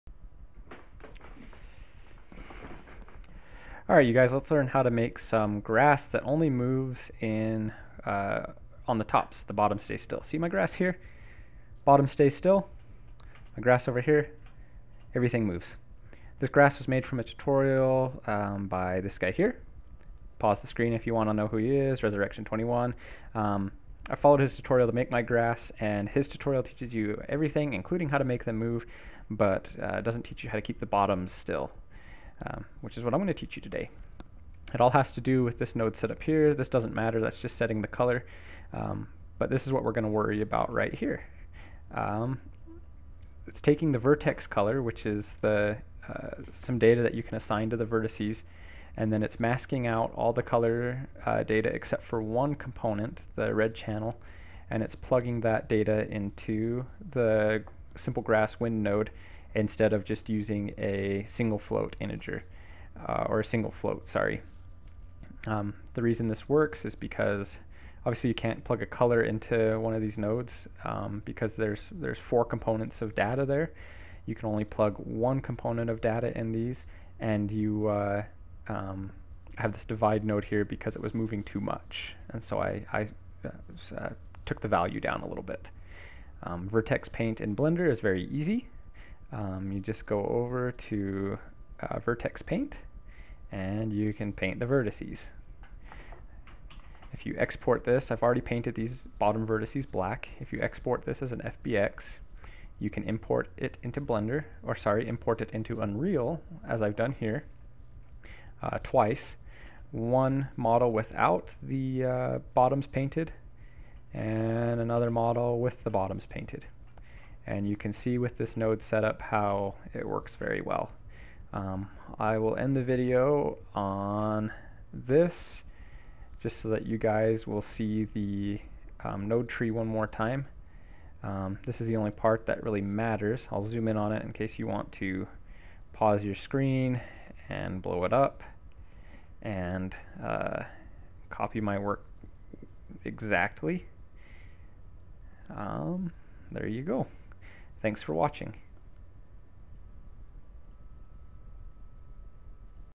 How to Keep the Bottom of Grass From Moving in Unreal
Foliage tutorial for Unreal from Blender